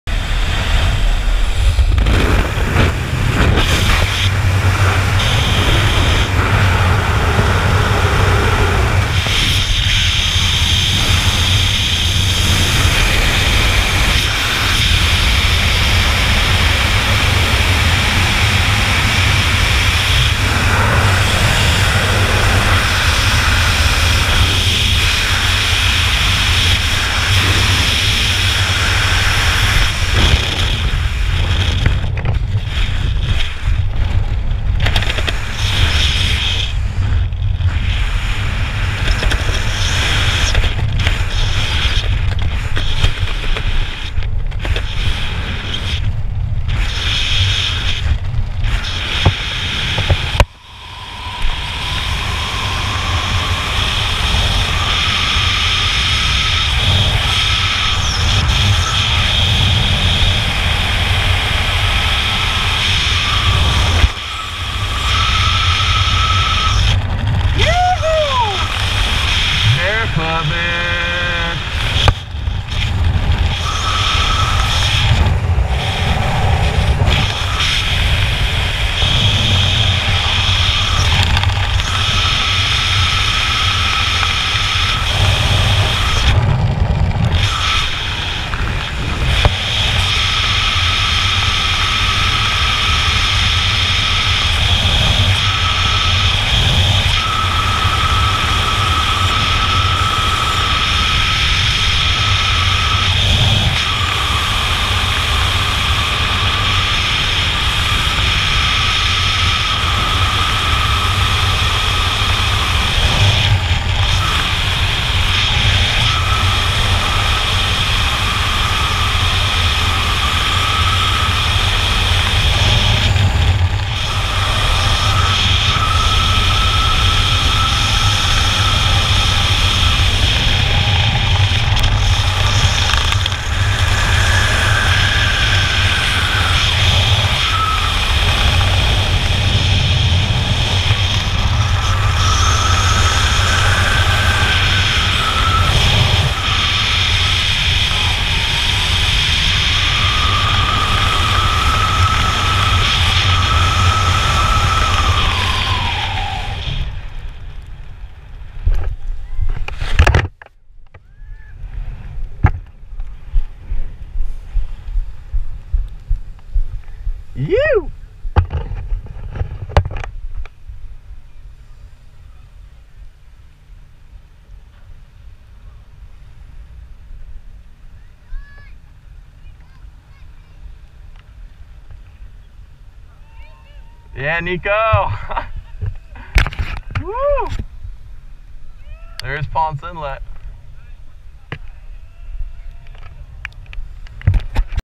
SKYDIVING // BEACH JUMP // TRACKING OVER THE OCEAN // [GoPro HD] 2017
Skydiving over the beautiful beaches of Florida. This is my skydive beach jump from "disappearing island jump 2017"!! What an awesome time, and an awesome party on the ground!! There was live music, food, and lots of good times!!! What an awesome day this was!!